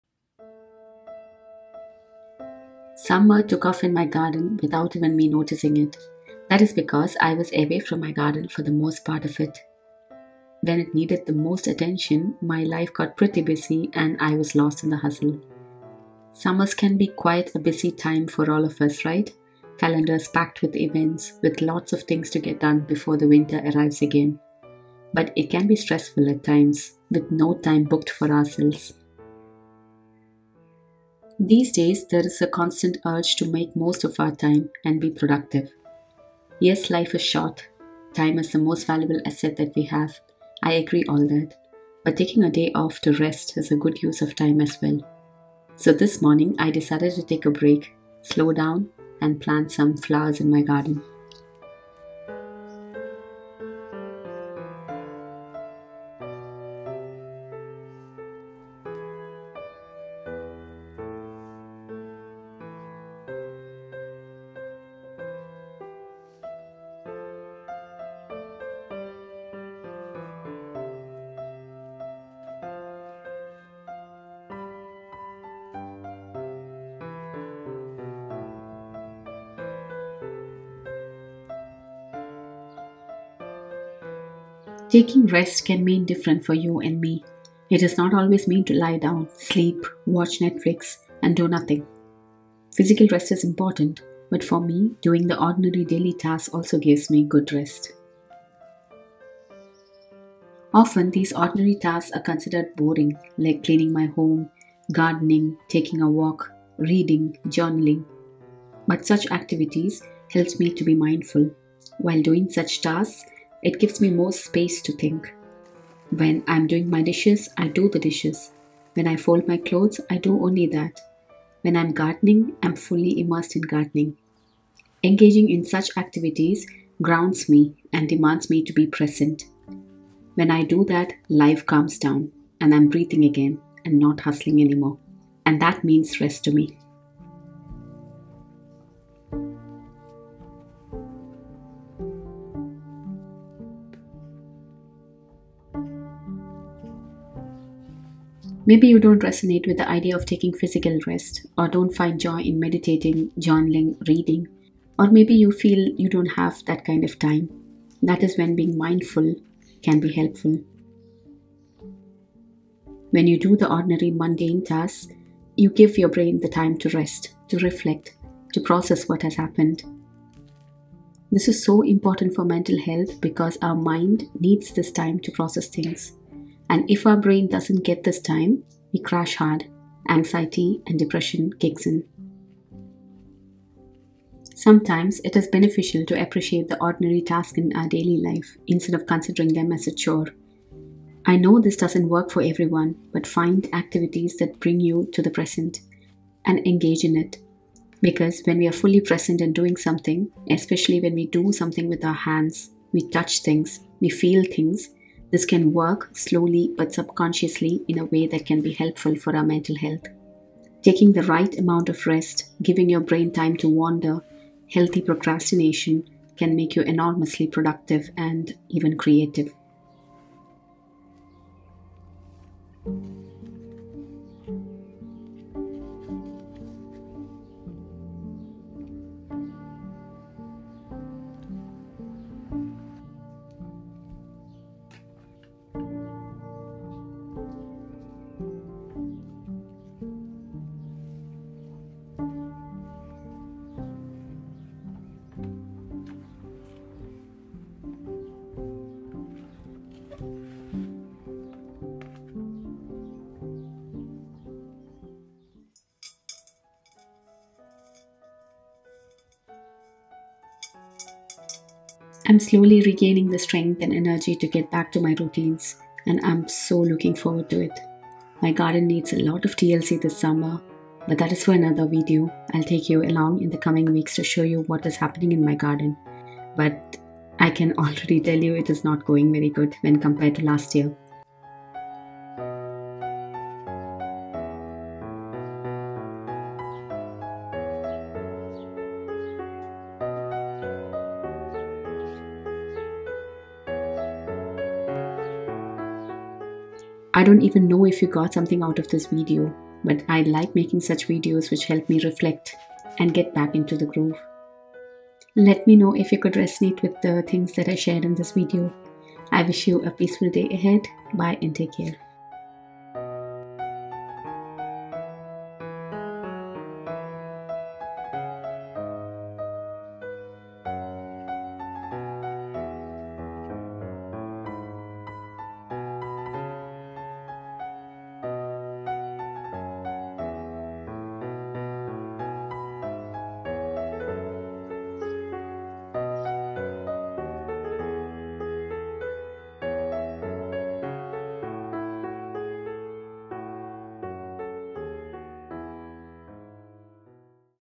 Planting Flowers in my Garden | Taking a Break
This video is not usual gardening video that I post on my channel. Here I talk about the importance of taking rest, define what rest means to me while planting some flowers in the garden. Hope you enjoy!

My filming equipments:
Small Tripod

Happy Gardening!
Aparna